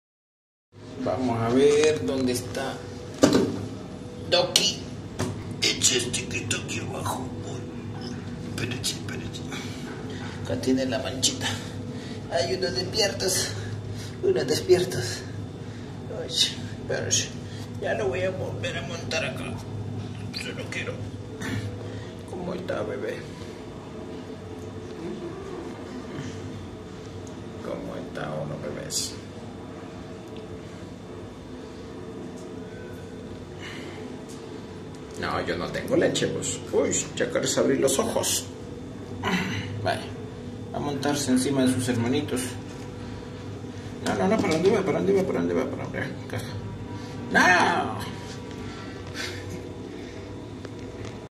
Beagle TGU » Cachorros Beagles (Doky)
Hola le saluda Ramón Figueroa Criador de mascotas, cachorros de la raza Beagle.

Nuestros cachorros Beagle están a la venta en Tegucigalpa, Honduras, Centro América.

Los entregaremos con 3 de sus 6 vacunas recomendadas, les aplicamos (parvovirus, corona virus y moquillo) y los desparasitamos.

La edad ideal para traer un Beagle a casa sería de al menos dos meses, cuando come por si mismo.

El peso de un Beagle oscila entre los 10 y 18 kilos.

Respecto al color, se admiten tricolores (café, blanco y negro) y bicolores (café y blanco) El manto es corto, denso y resistente al clima. Tienen orejas largas y pegadas al cuerpo.